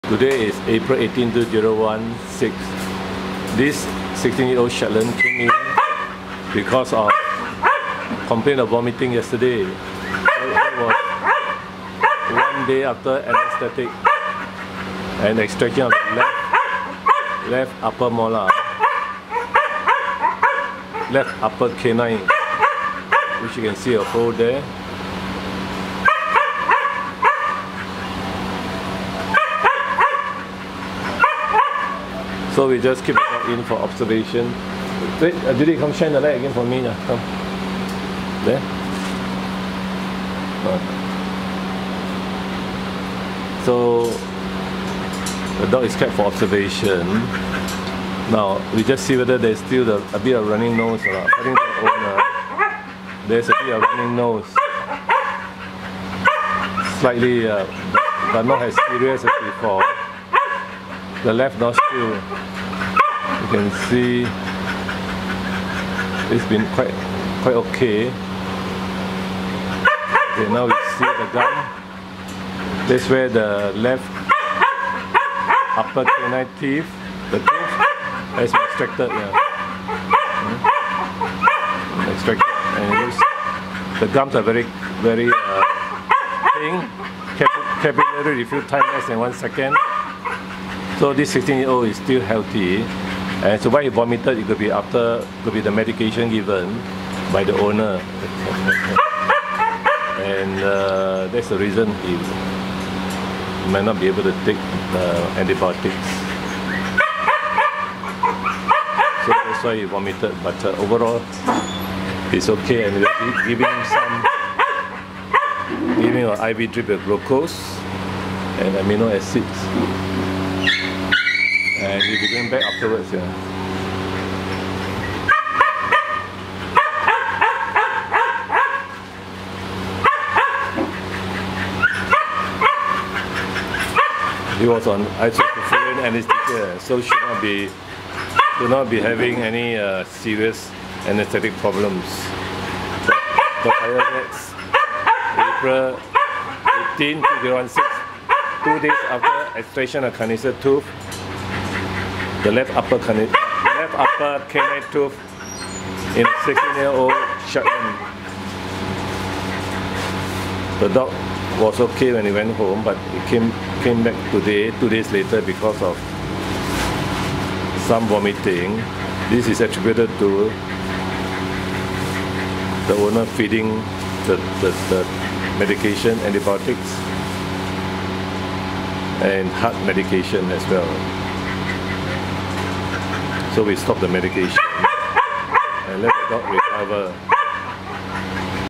16-year-old Sheltie vomited yesterday after left upper canine extraction,
2 days after extraction of the left upper canine, the Sheltie vomits. However, the owner is very happy that there is no more yellow thick mucoid left nasal discharge. The vomiting could be due to the effects of intubation irritation, vaccination given or medication. The dog is very old and it is hard to say what causes the vomiting post surgery. A possible cause could be the painful mouth after tooth extraction or water in the lungs from heart disease.

Blood test showed no liver or kidney impairment.

An IV drip is given and he is active again.

Follow up on Apr 20, 2016. No more vomiting. I advised the owner to give small amounts of canned A/D, honey and water. The owner is advised to continue the heart medication.  She is just so happy that there is no more left nostril discharge which has been persisting for at least 6 months. This was due to the pus from the  canine root abscess entering the nostril area.